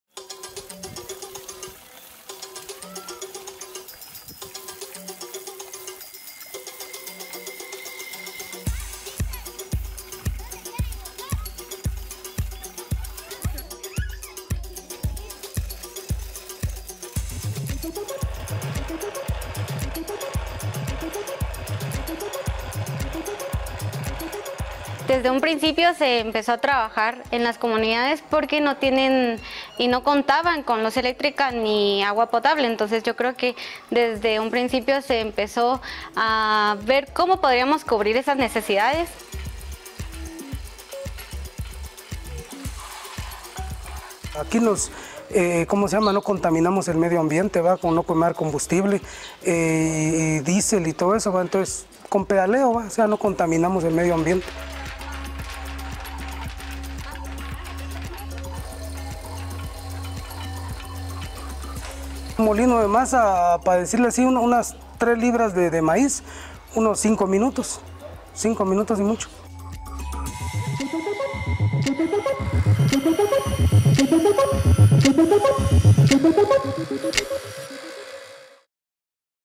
Guatemalan indigenous people recycle bicycles into machines
From pumping water from a well to shelling a corn, a group of Guatemalan indigenous people adapt recycled bicycles into machines that help protect the local environment and support poor families. (AFP)